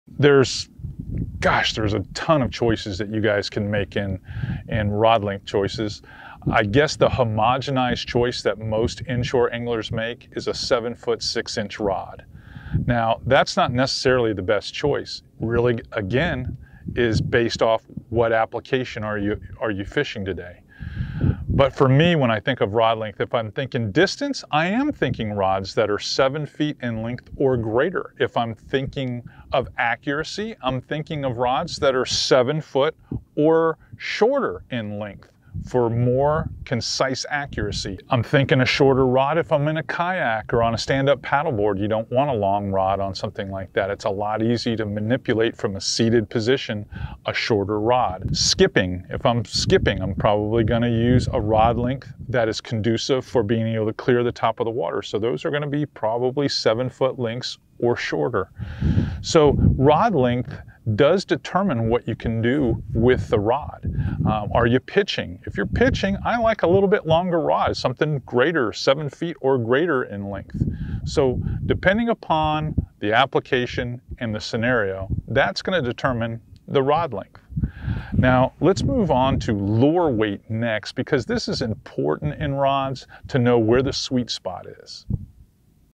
Rod Length Suggestions - Flats Class University
Rod length is determined by the scenario. For example, are you long casting or going for accuracy? In the comments below, let me know if this tip was helpful to you!